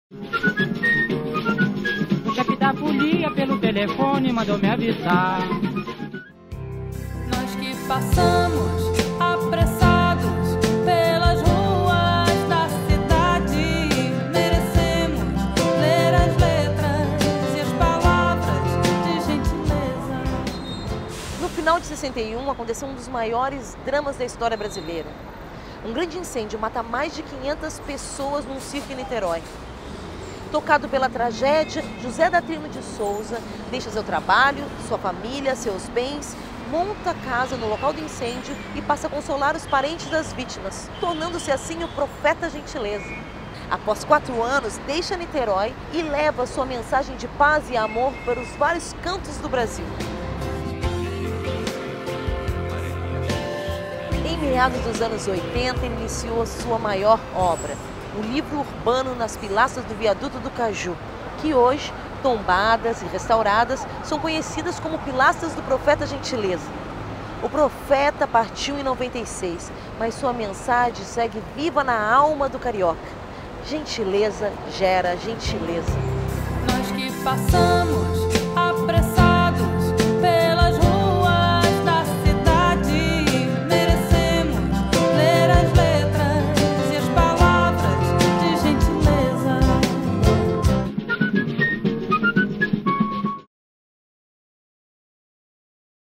Gentileza
"Gentileza gera gentileza". Que carioca não conhece esta frase simples, mas que resume toda uma filosofia de vida? Hoje o Porto EnCantos traz mais um dos EnCantos da Região Portuária do Rio, o Livro Urbano de José Datrino de Souza, o Profeta Gentileza! e